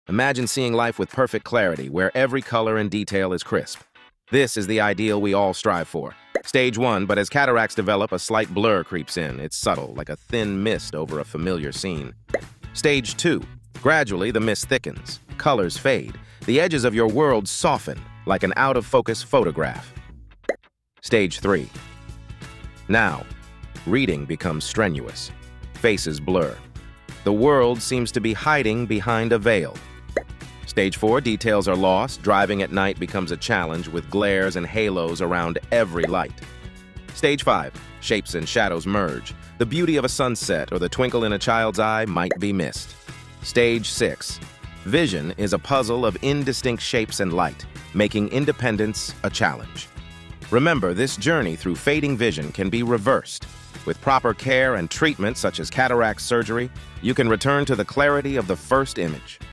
Cataracts Vision Simulation
This video brings you closer to understanding how cataracts cloud your vision, gradually taking away the clarity of your sight. This eye condition slowly dims and blurs the vibrant details of the world around you, making everyday activities challenging.

In our video, we dive into the early signs of cataracts and their development over time, emphasizing how they impact daily life, from struggling to read small print to difficulties in low light. We'll walk you through the journey of recognizing and managing this common condition, sharing effective strategies for maintaining eye health and slowing progression.

Learn about the transformative power of cataract surgery, offering hope and a brighter, clearer future for those affected. Plus, discover preventative steps you can take today to protect your eyes and potentially delay the onset of cataracts.

Whether you're interested in identifying cataract symptoms, exploring options for correction, or understanding more about the condition's effect on vision, this video provides all the essentials. It's designed to demystify cataracts, offering insights into treatment and the importance of eye care. Tune in to empower yourself with knowledge and take the first step towards clearer vision.